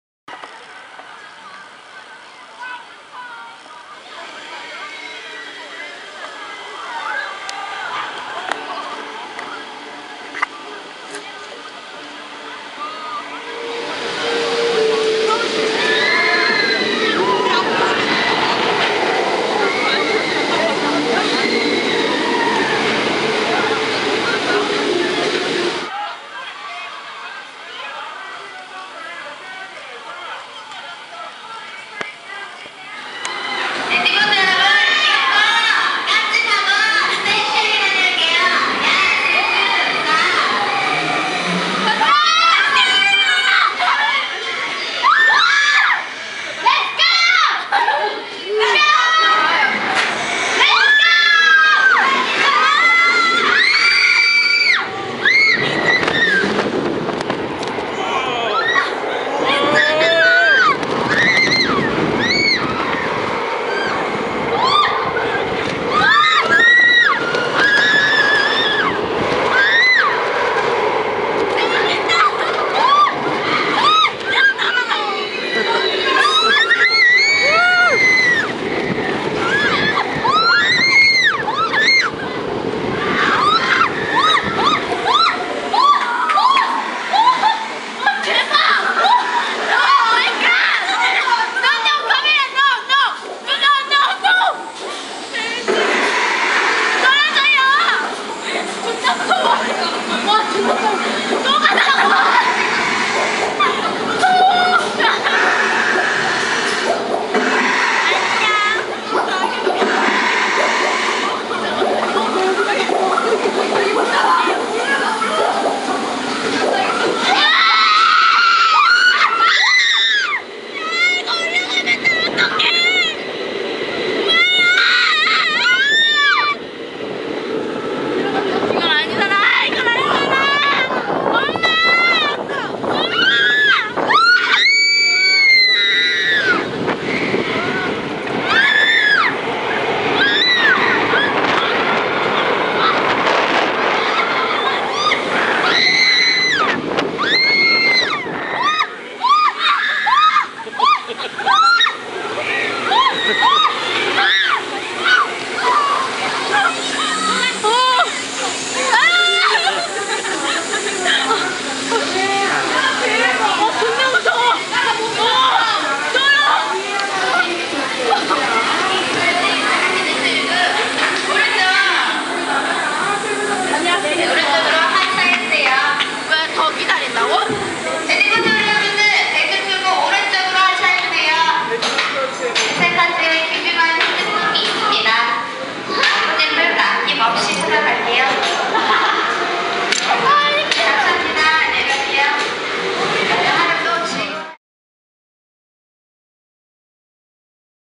롯데 월드 한국 Atlantis Adventure Roller Coaster POV FRONT Lotte World South Korea
Take a POV ride on the "unique" Atlantis Adventure Launch Roller Coaster at Lotte World in South Korea!

Manufacturer: Intamin
Opened: 2003
Length: 2198.2
Height: 72.2
Inversions: 0
Speed: 46.6 mph
Duration: 1:48

This very unique water coaster has no splash down sections, the track just has water sprayers that spray the side of the car. You should check this one out, It's one of the unique and rare and bizarre coasters.